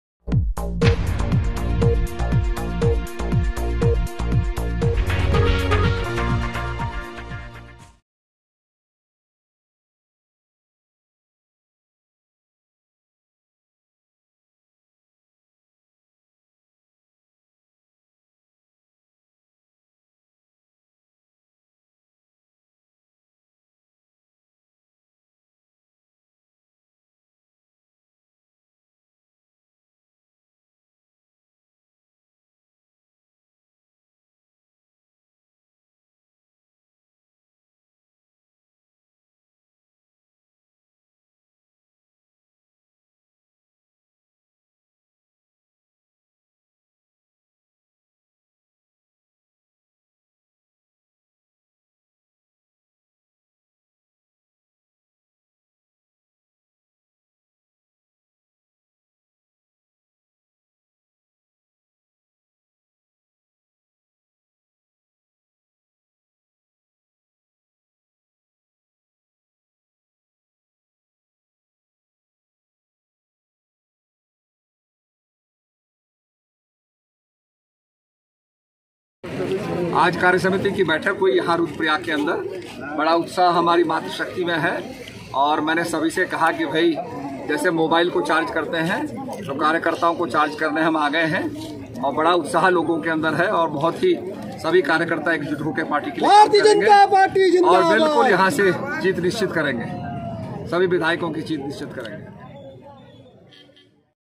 कार्यसमिति की बैठक में भाजपा कार्यकर्ताओं में महाराज ने भरा जोश, कहा - मैं आपको चार्ज करने आया हूं
उत्तराखण्ड: कार्यसमिति की बैठक में भाजपा कार्यकर्ताओं में महाराज ने भरा जोश, कहा - मैं आपको चार्ज करने आया हूं